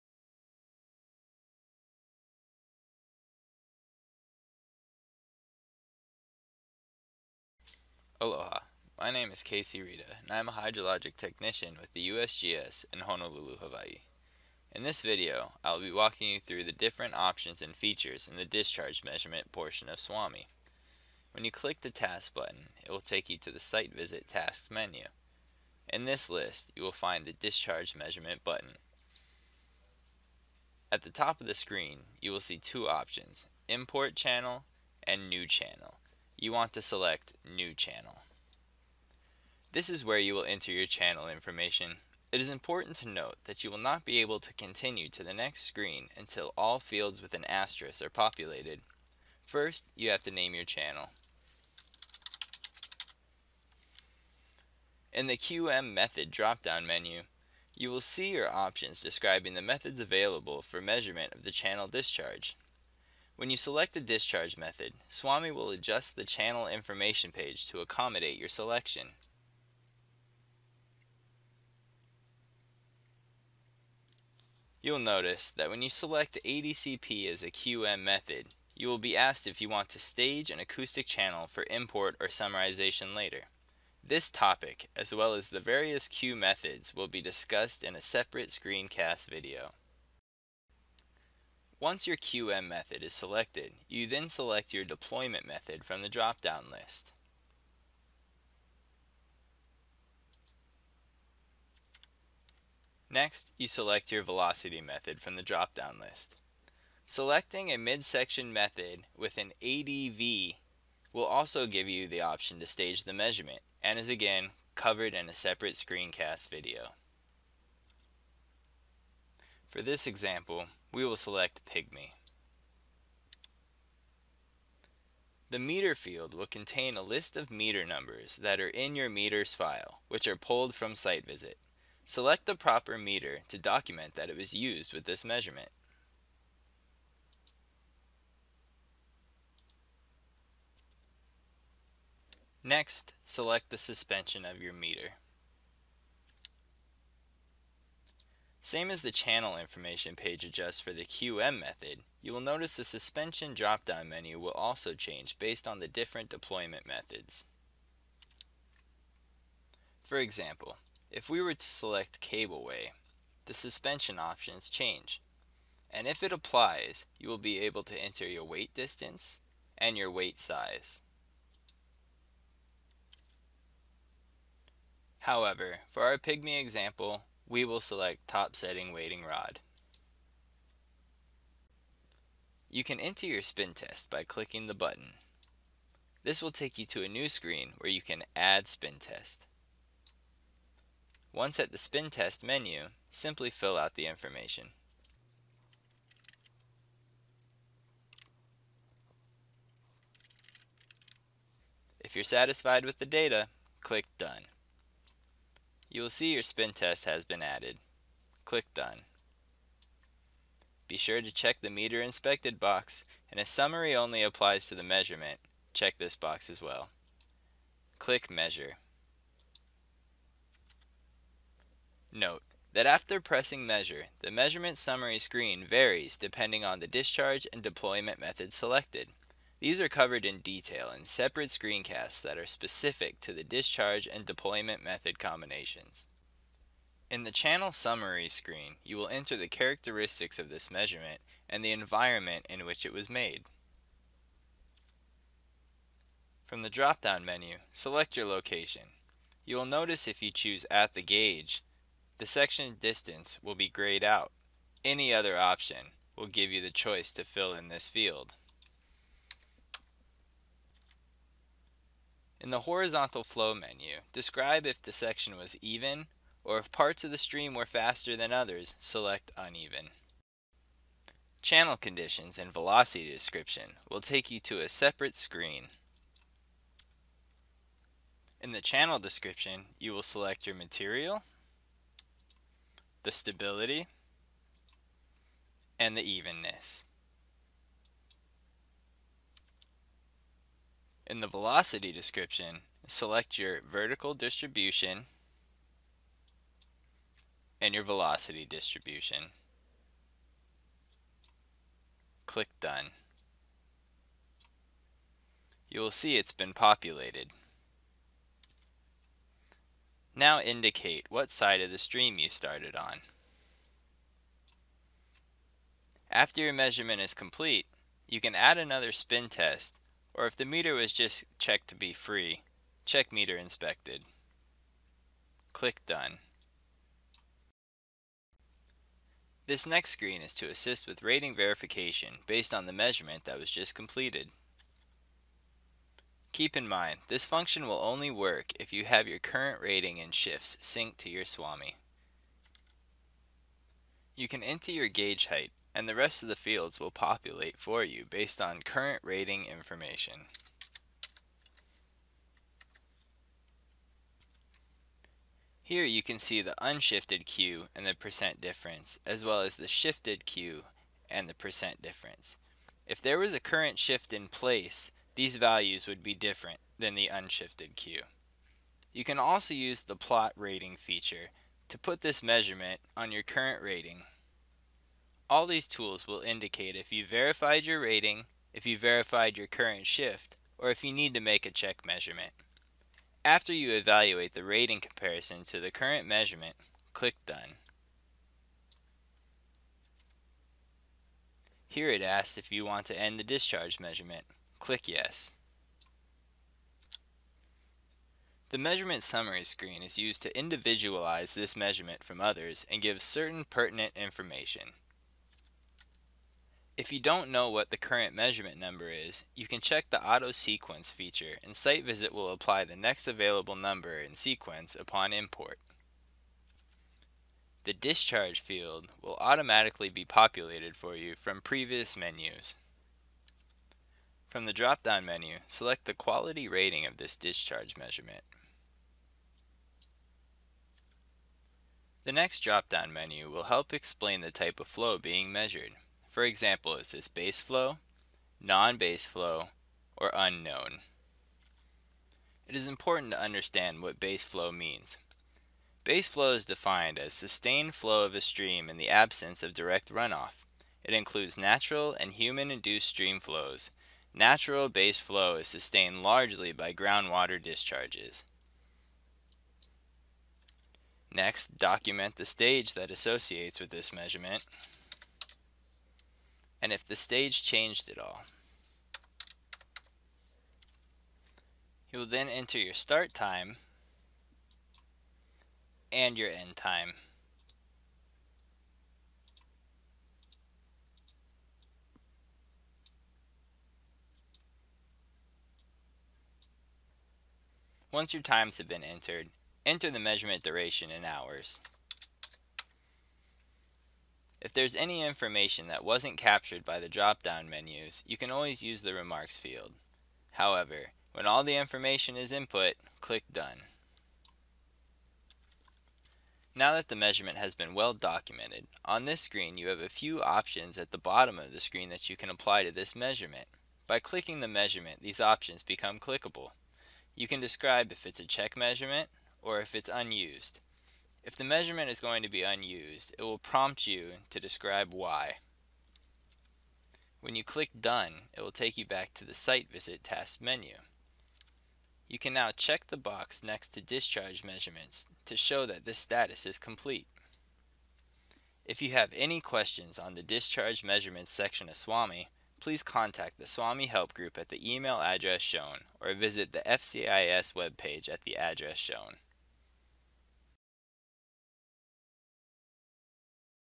5 Discharge Measurement-General Information
This video provides an overview of the discharge measurement task in SWAMI.